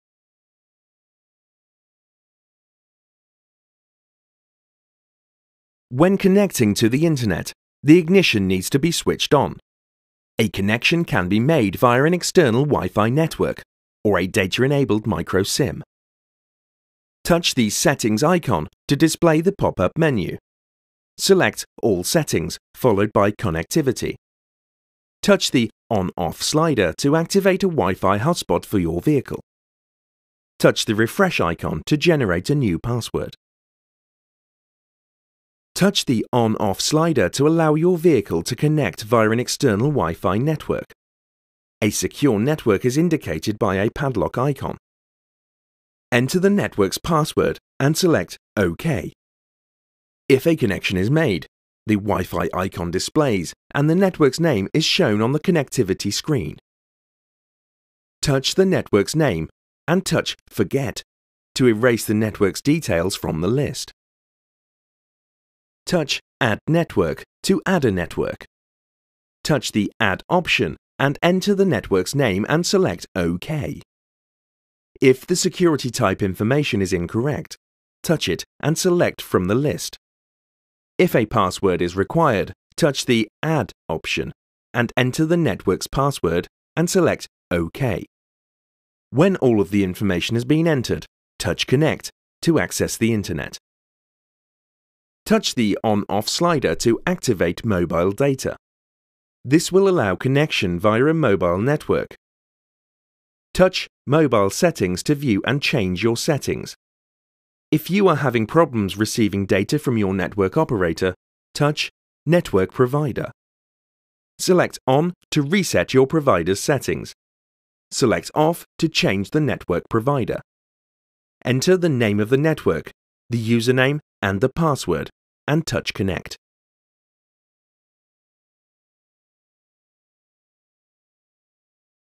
Range Rover Velar- Touch Pro Duo Connectivity
Learn how to operate the Range Rover Velar Touch Pro Duo Connectivity. For more instructional information, please visit our YouTube page or our